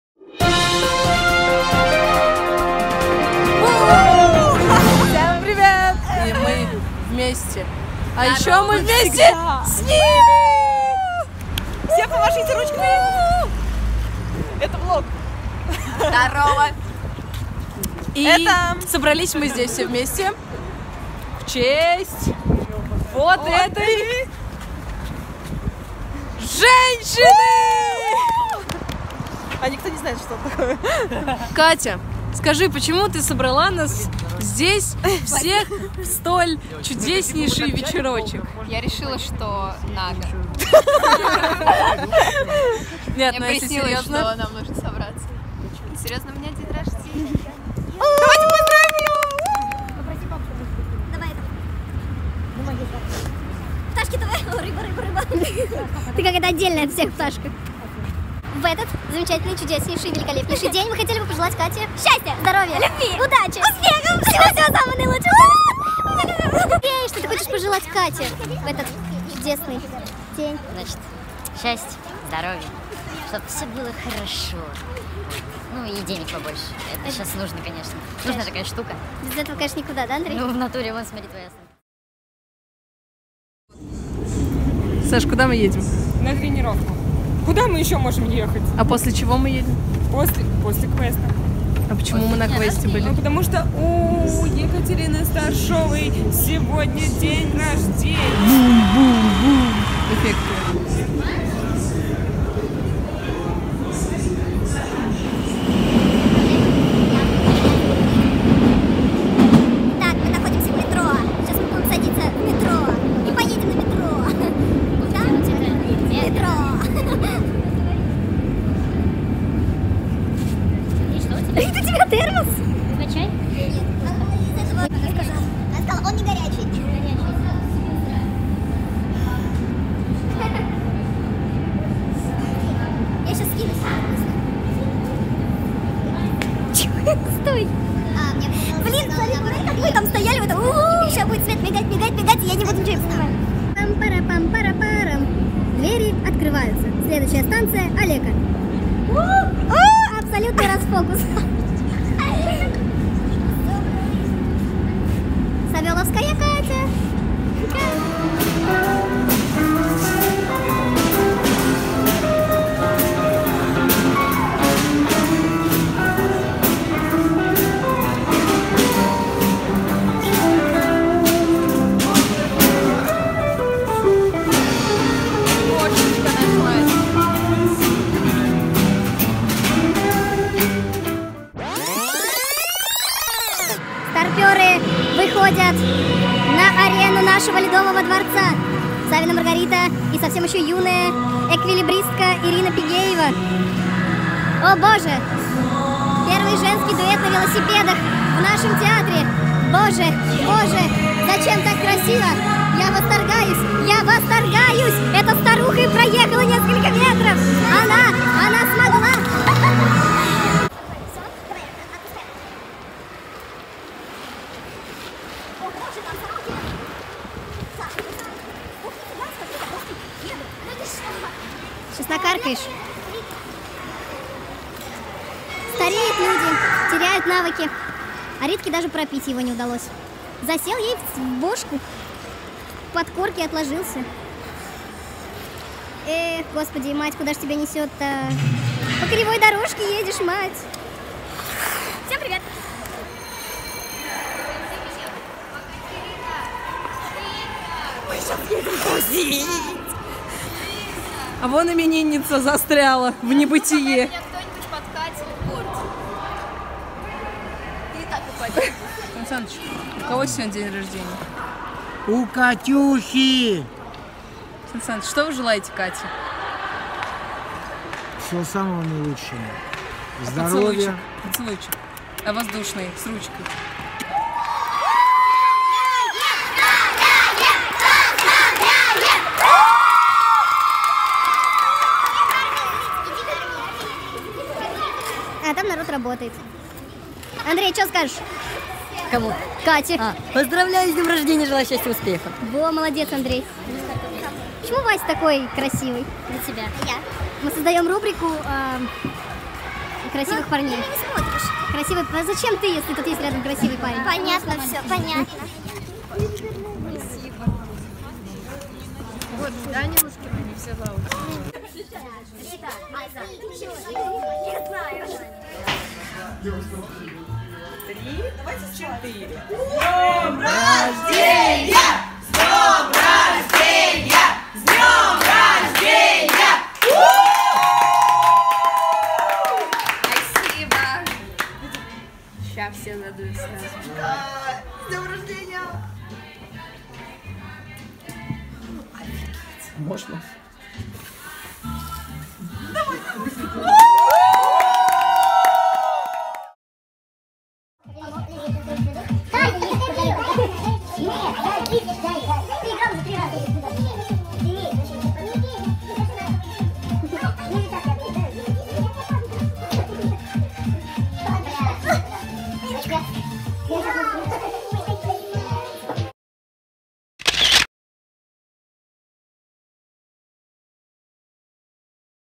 VLOG: Birthday party 🎉II ШОК ПУГОВКЕ 16!!! II Страшный квест и Антикафе 👻
Кате Старшовой 16!!! День рождения Папиной Дочки❤️👧🏽 Конечно же Пташки не смогли пролететь мимо этого праздника🐣💙🐣